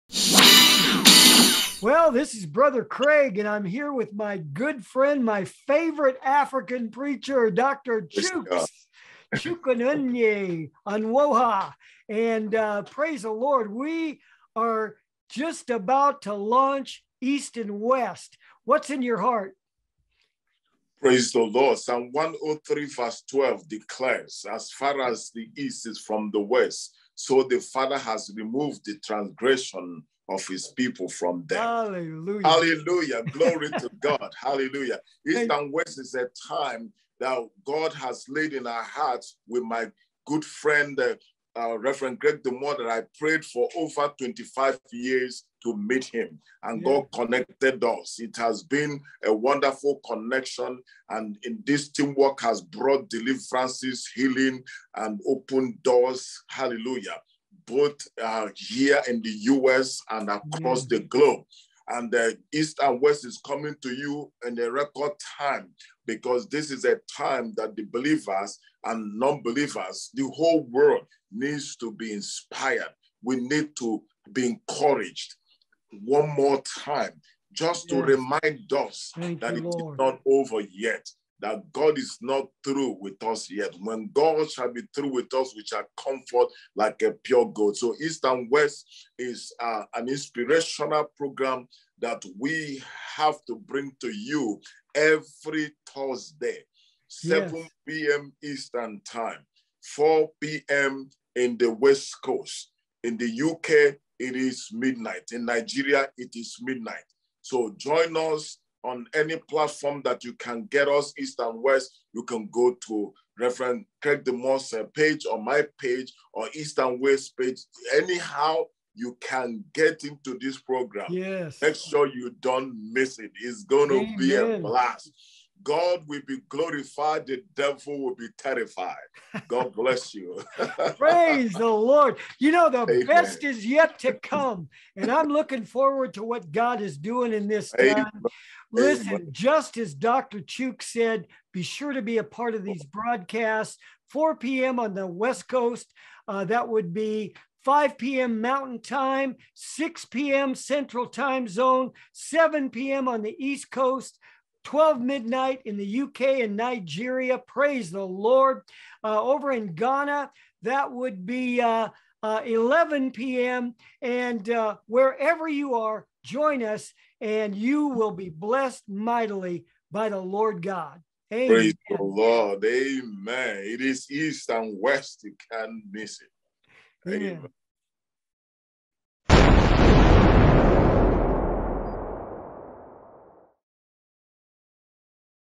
JOIN US for East & West!
JOIN US for “East & West” every Thursday @ 4 pm Pacific, 7 pm Eastern, 12 midnight UK/Nigeria – your regular dose of encouragement and uplift.

Ways to access the broadcast:
Facebook -- East and West Fire Chukwunenye Onuoha Craig DeMo and more. More sites to be announced.

✔️Visit AmbassadorMinistries.org
Remember that if you're a child of God, you are God's ambassador! You're His representative on the earth. 2 Corinthians 5.20.